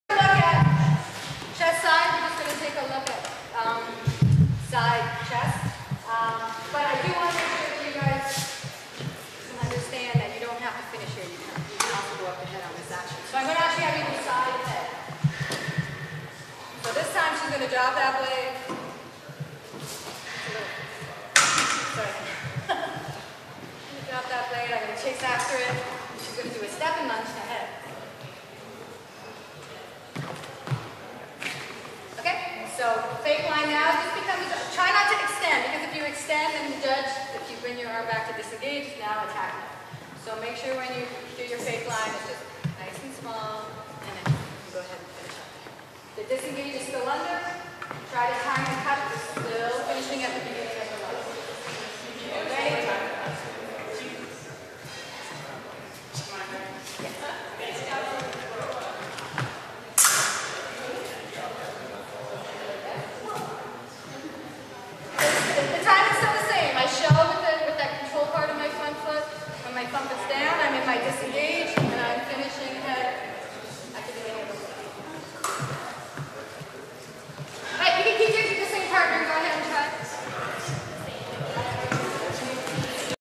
Ontario Fencing Association Jr HP Sabre Camp May 31, 2014 Video 10
The Ontario Fencing Association Jr HP Sabre Camp was held at Bladeworks Fencing Club in Toronto. Carolyn Washburn from Beaches Fencing Club NY guest coached